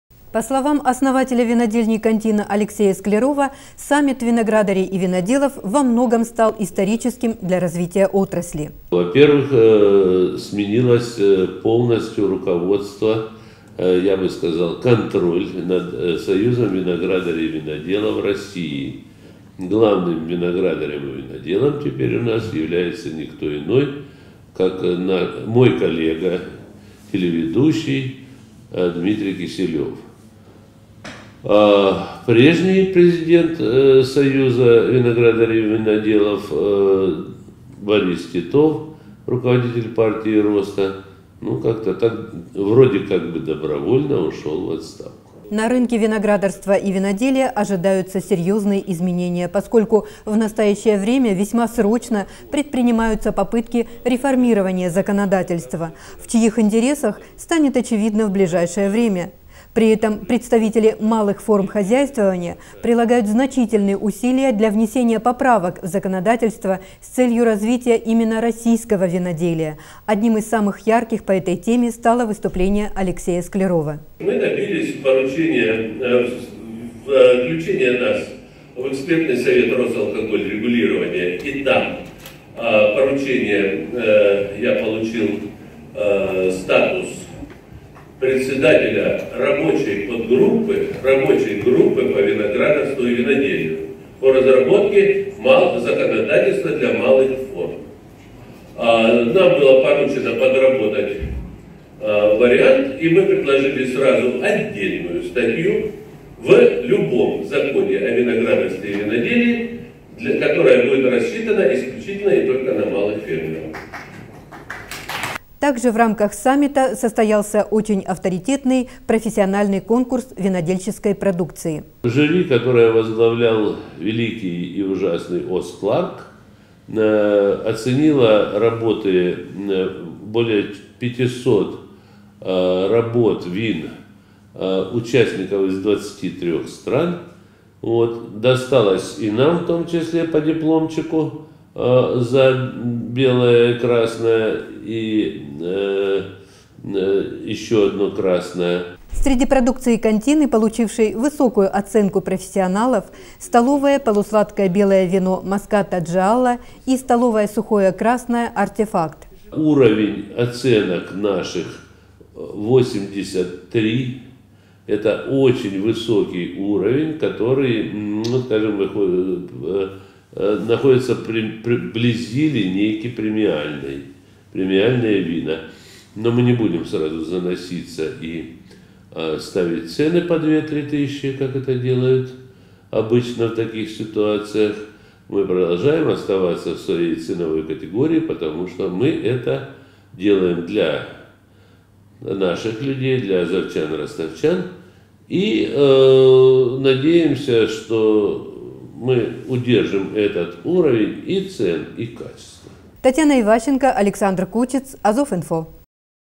Кантина получила высокую оценку в Крыму
В минувшие выходные в Ялте на территории винзавода «Массандра» состоялся IX Всероссийский саммит виноделов. Крым впервые принимал профессиональный форум, собравший огромное количество  участников, включая авторитетных зарубежных экспертов. В работе престижного форума участие приняли и  виноделы азовского фермерского хозяйства «Кантина».